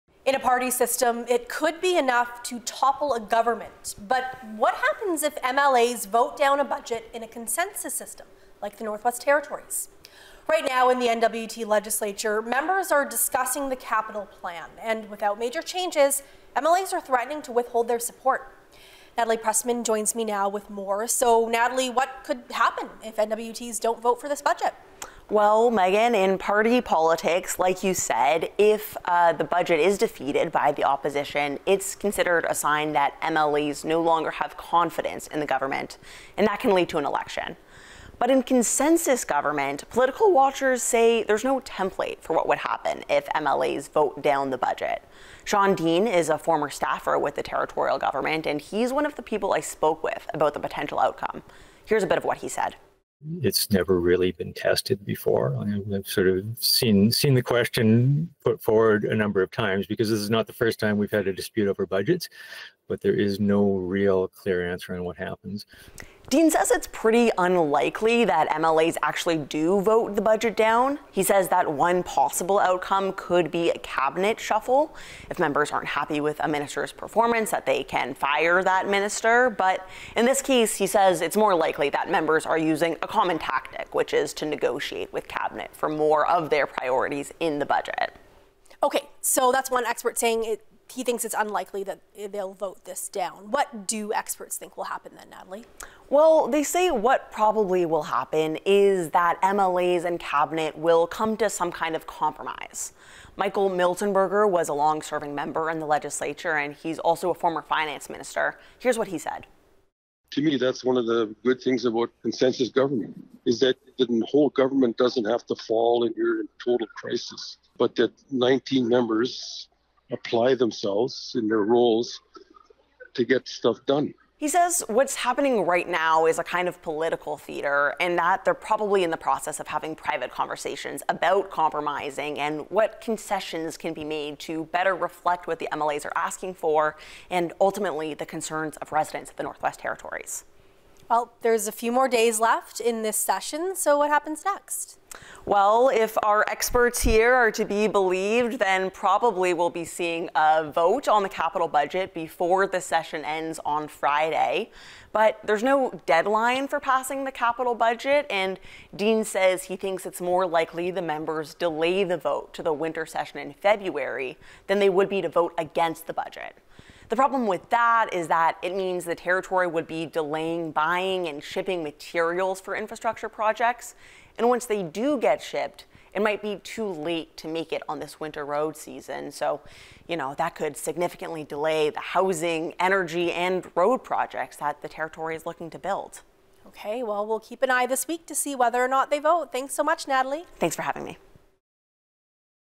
What happens if N.W.T. MLAs vote down the budget?
In a party system, it could be enough to topple a government. But what happens if MLAs vote down a budget in a consensus government? The CBC's Natalie Pressman breaks it down.